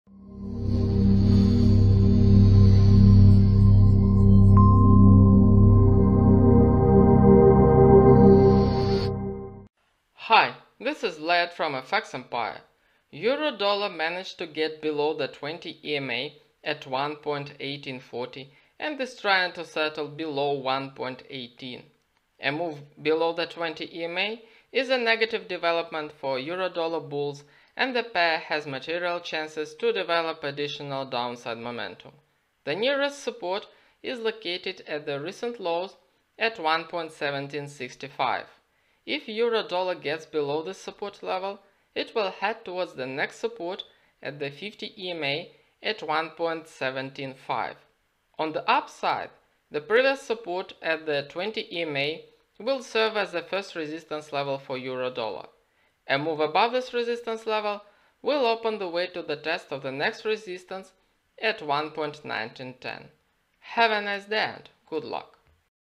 EUR/USD Technical Analysis For September 3, 2020 By FX Empire
EUR/USD managed to get below the 20 EMA at 1.1840 and is trying to settle below 1.1800.